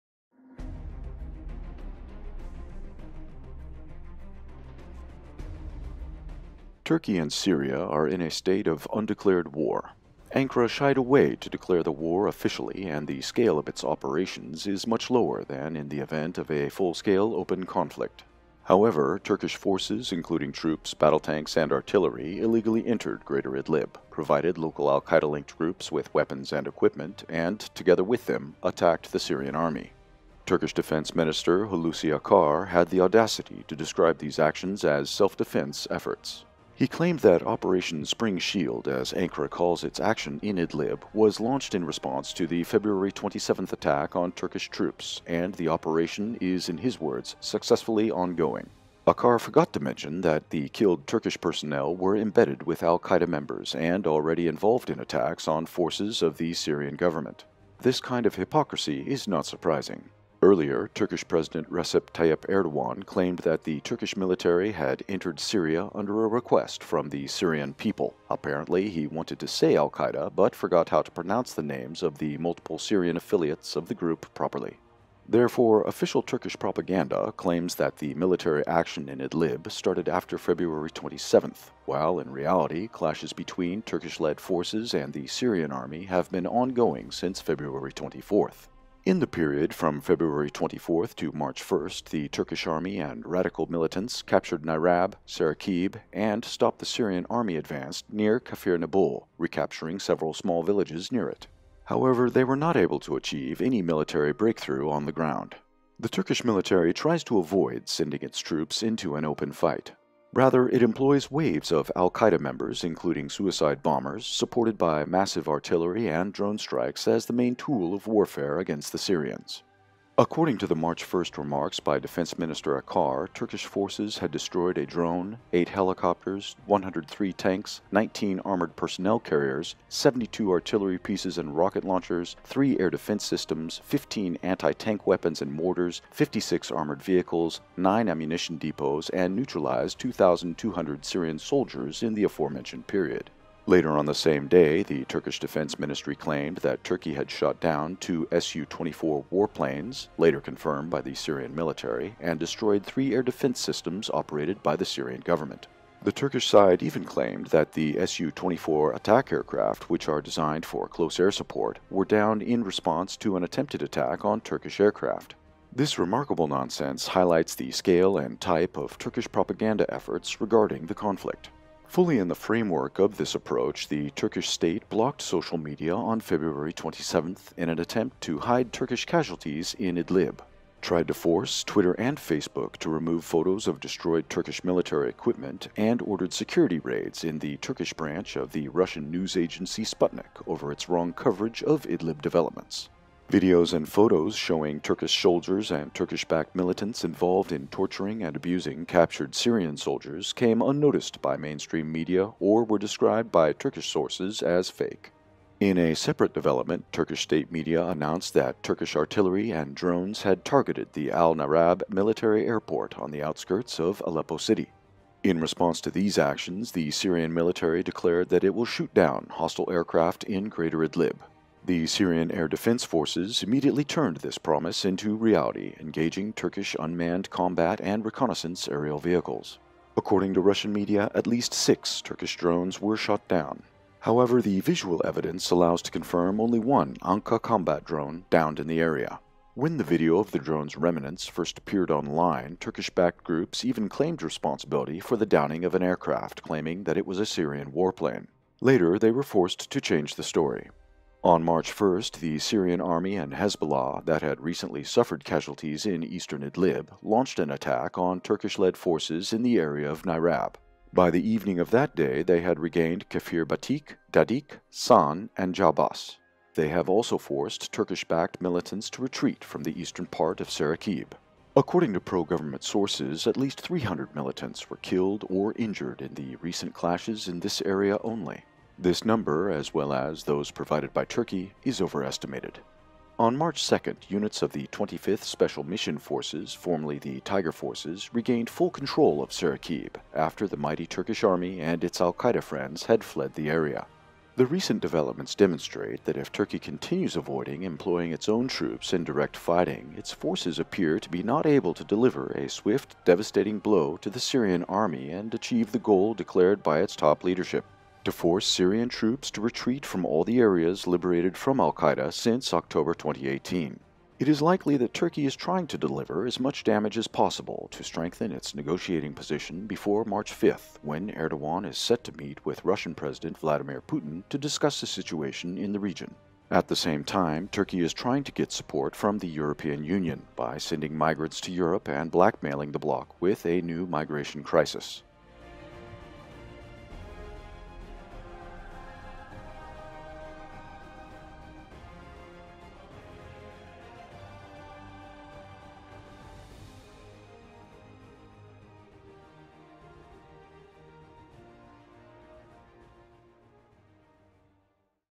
SAA recaptures Saraqib from Erdogan-led alQaeda gangs
Second liberation of Saraqib and discussion of the Turkish Government role. Video from South Front, 3 March 2020, original post